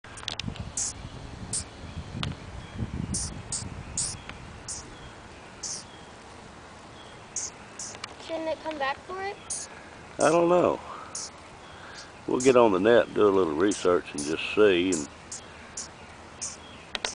Baby Opossum dropped by Mother during release.
We put the baby in a small box and put it by Mother Opossum's den. We will check this afternoon to see if Mom retrieved the baby. I catch Raccoons and haul them off as they are extremely destructive to my property.. I kill skunks because they get under the house and stink it up so I keep them thinned out.